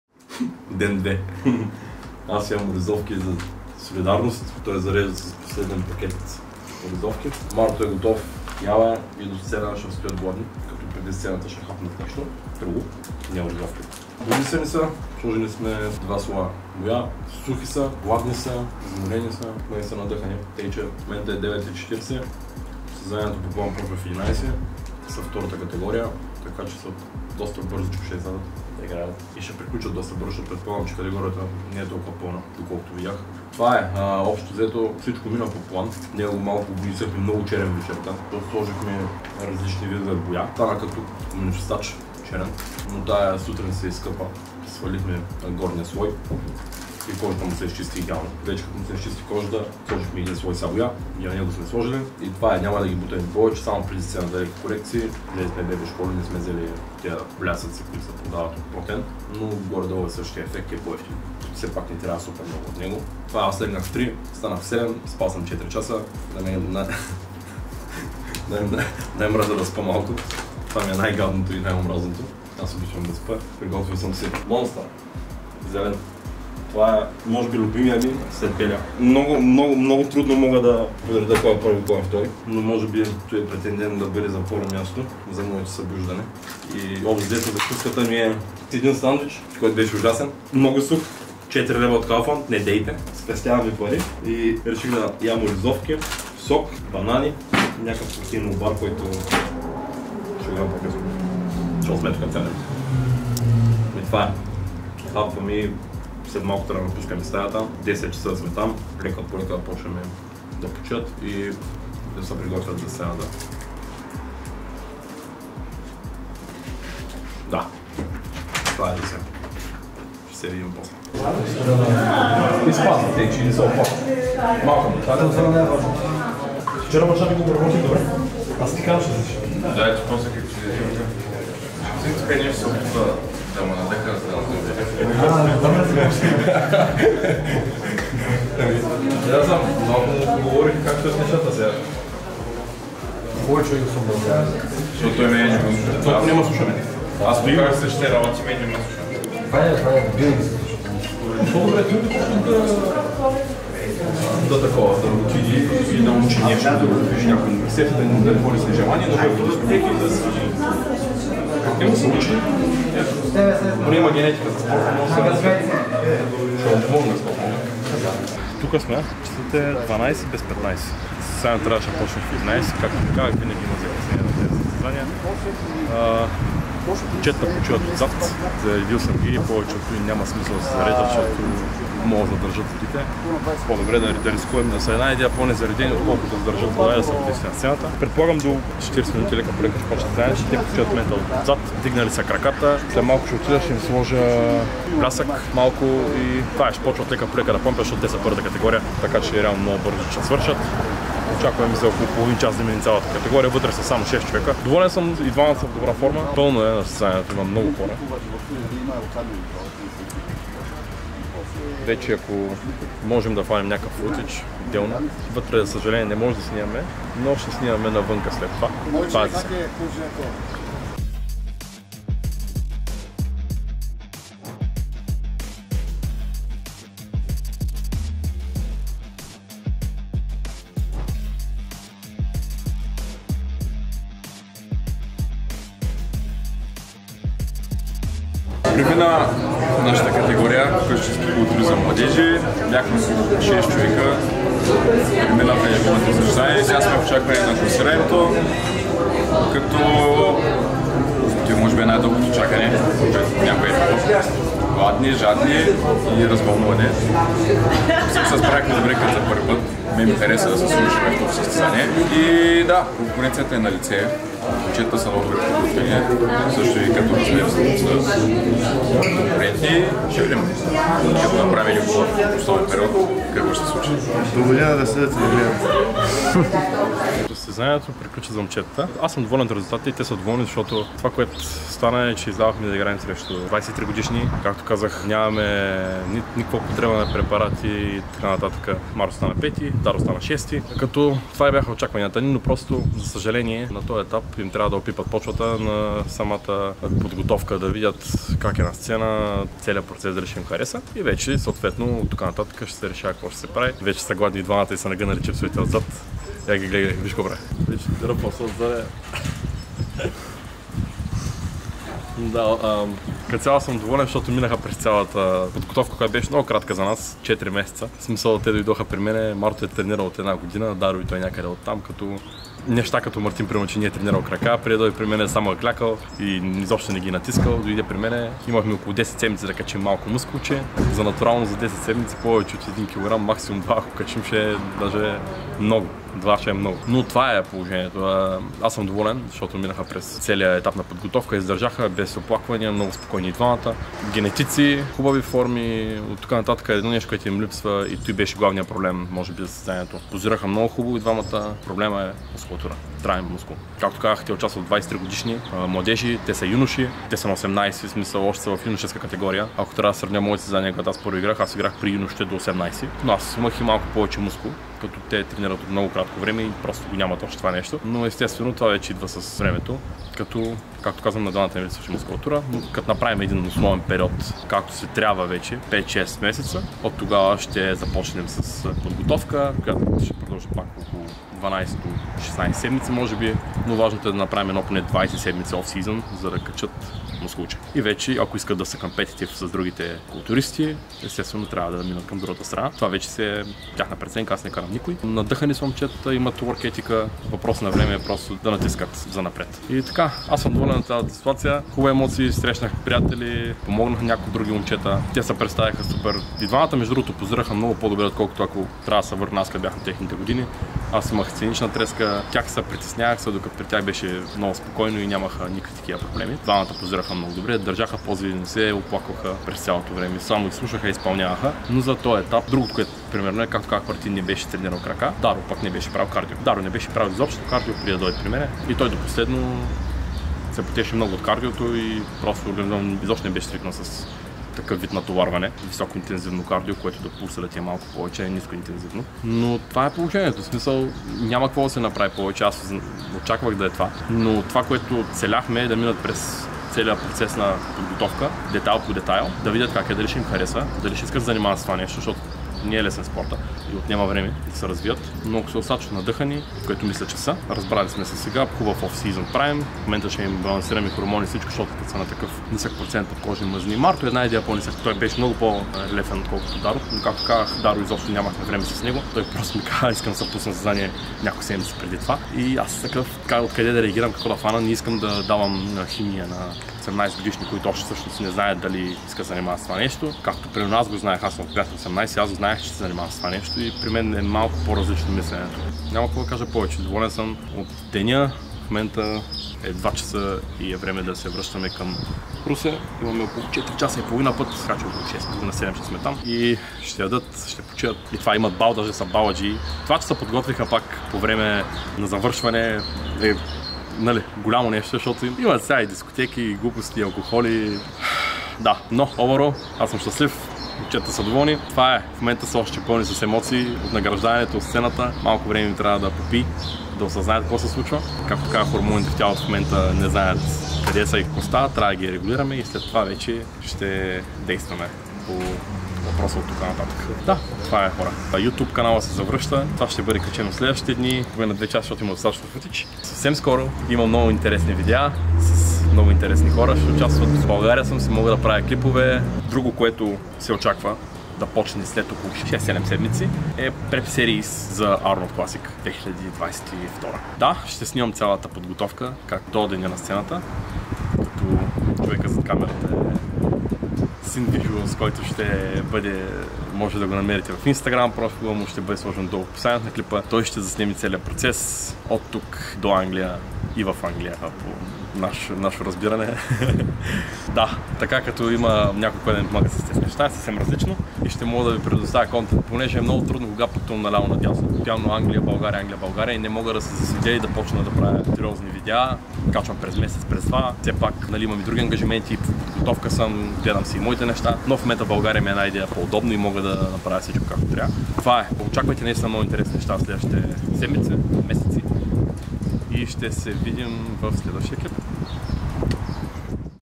ПАЗАРДЖИК - ДЕН 2 - СЪСТЕЗАНИЕТО, ARNOLD PREP SERIES
Ютуб се връща и съвсем скоро започва поредицата Arnold Prep Series.
Последвайте ме в инстаграм, ако не сте.
Също така и видеографа Sinnervisuals.